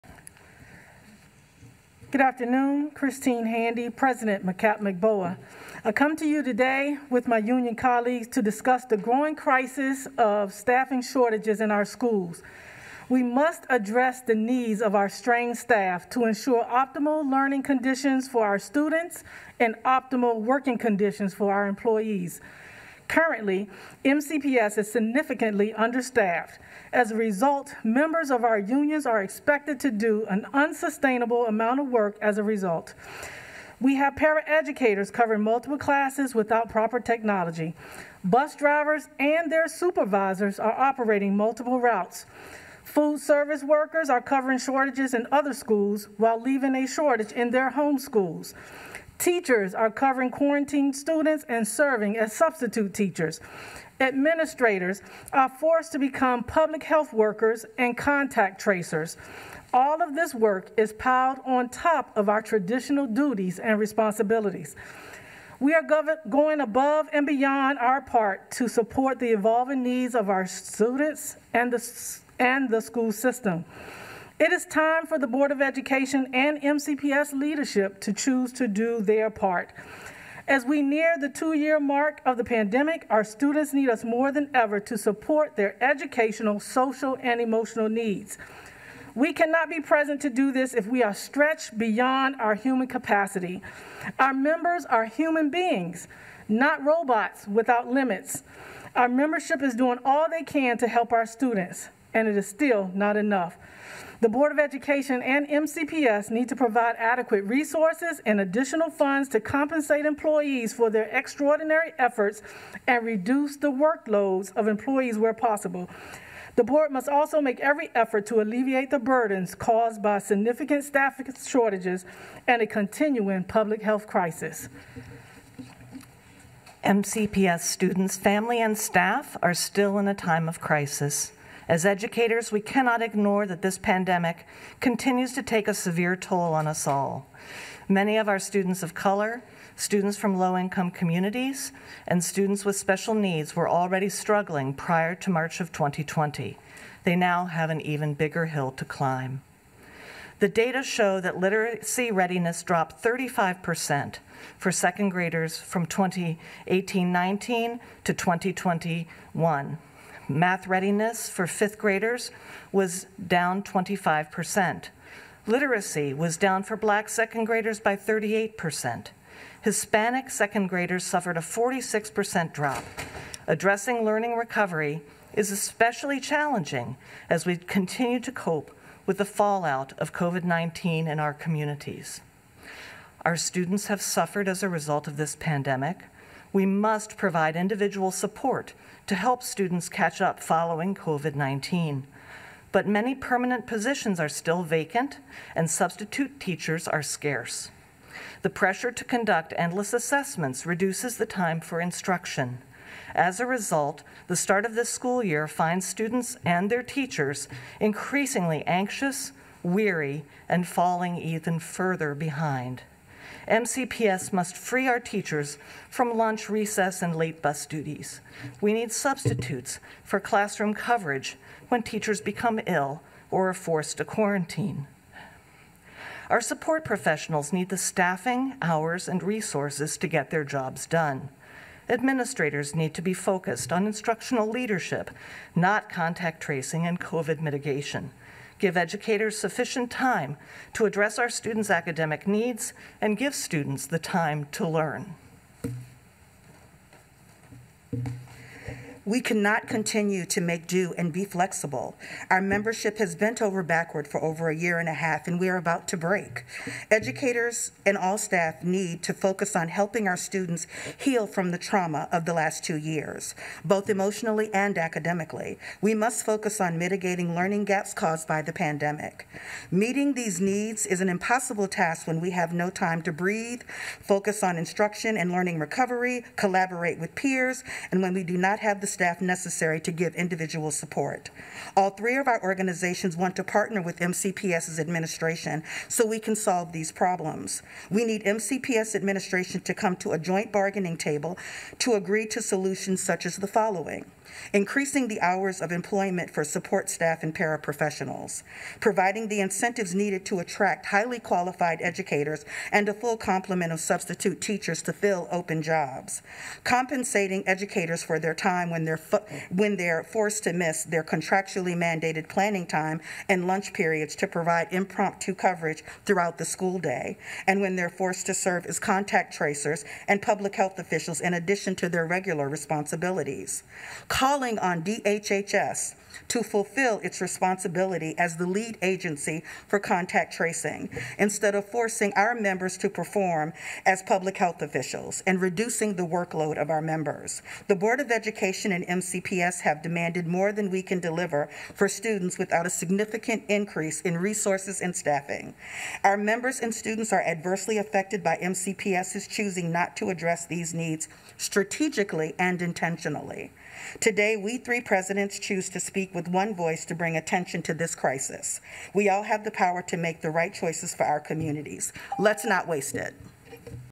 Union Presidents joint public comments at 10-5-21 MCPS BOE meeting
Union Presidents from MCEA, MCAAP, and SEIU Local 500 stand in solidarity in their call for MCPS to address the system-wide crisis of staffing shortages in our schools.